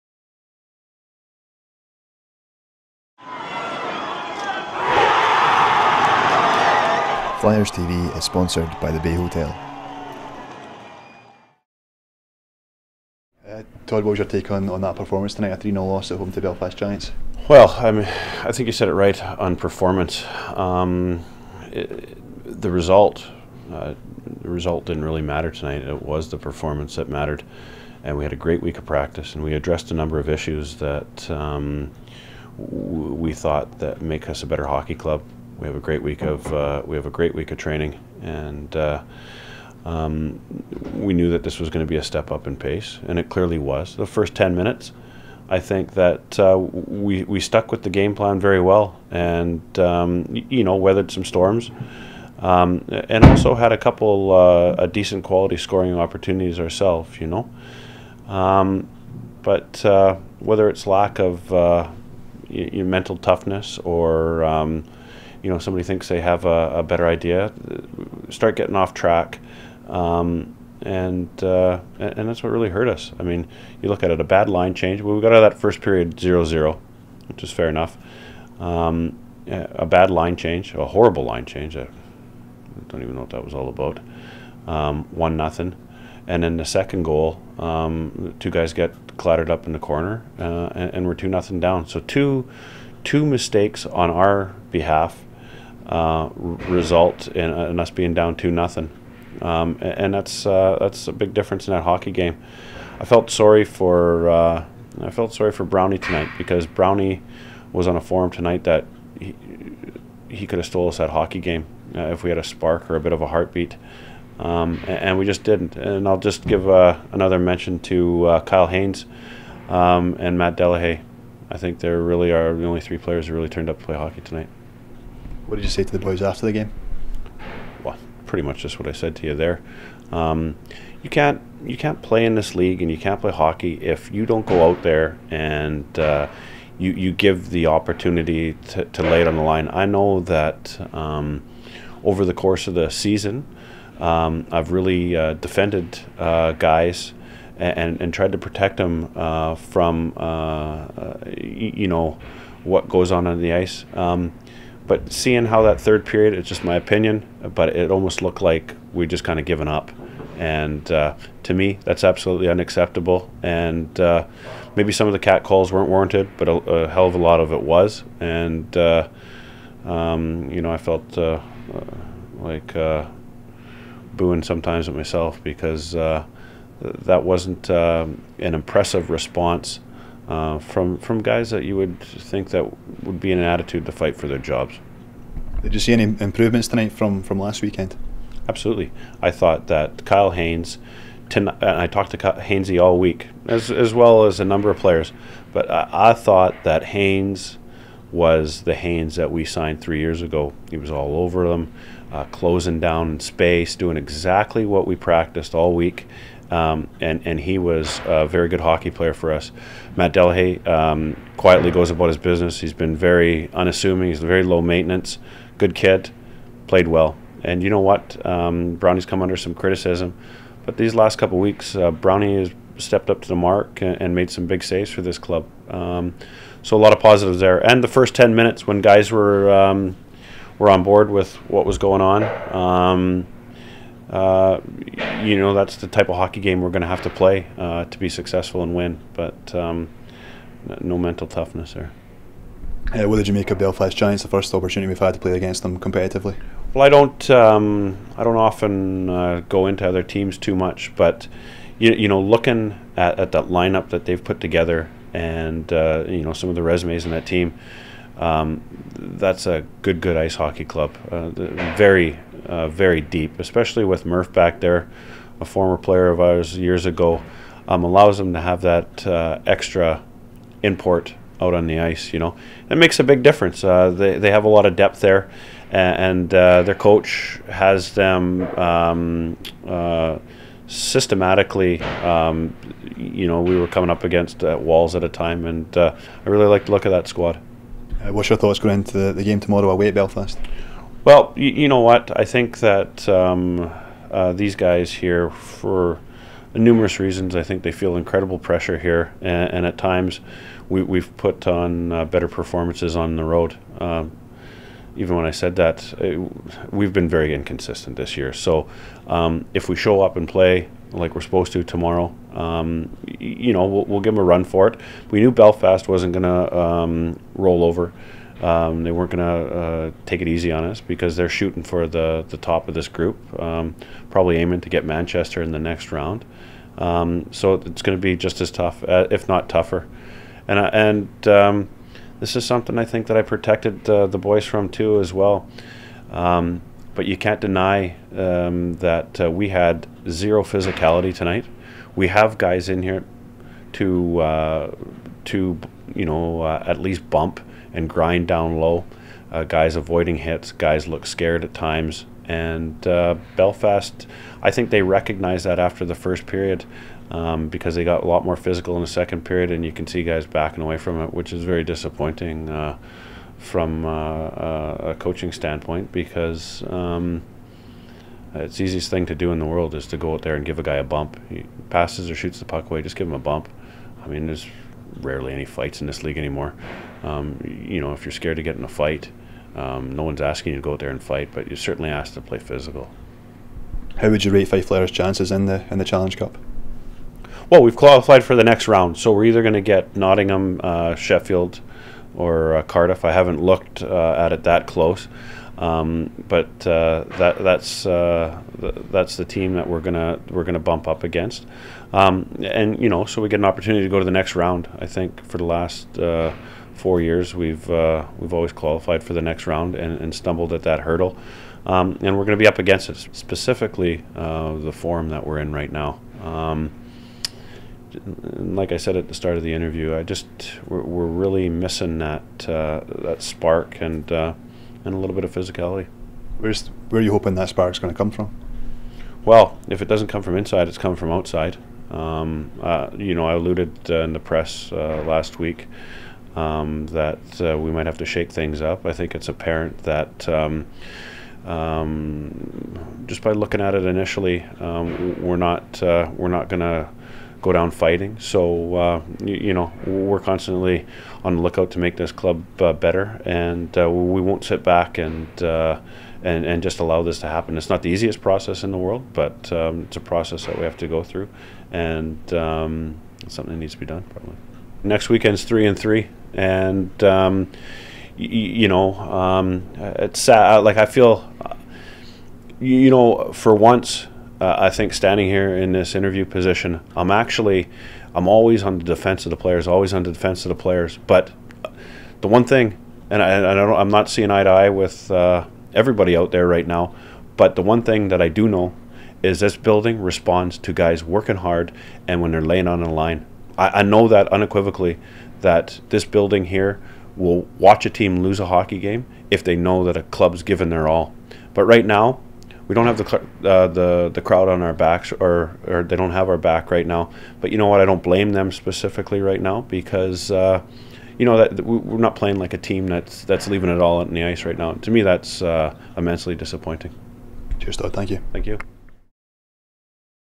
Todd Dutiaume on Challenge Cup loss to Belfast Giants
Fife Flyers head coach Todd Dutiaume speaks to the club YouTube channel following the 3-0 loss at home to Belfast Giants in the Challenge Cup.